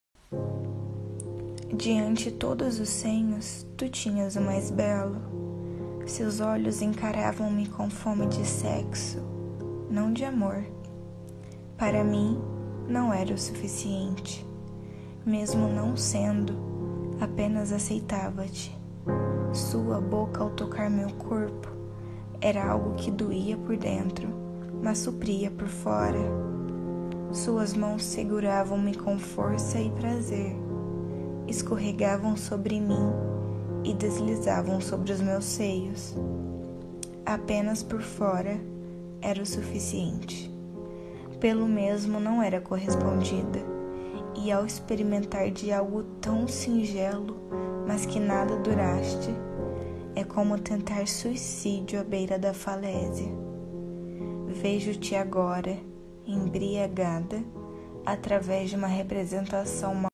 Arte para achegar - Maria Clara Rodrigues de Sousa (Poços de Caldas) declamando "não era pra ser"
“Arte para achegar “ retorna com Maria Clara Rodrigues de Sousa, aluna de Psicologia PUC Minas, em Poços de Caldas, declamando o poema "não era pra ser", de sua autoria. Clara é uma artista, seu poema está ilustrado em um lindíssimo clipe. Obrigada Clara! Envie-nos novas contribuições, serão sempre bem vindas.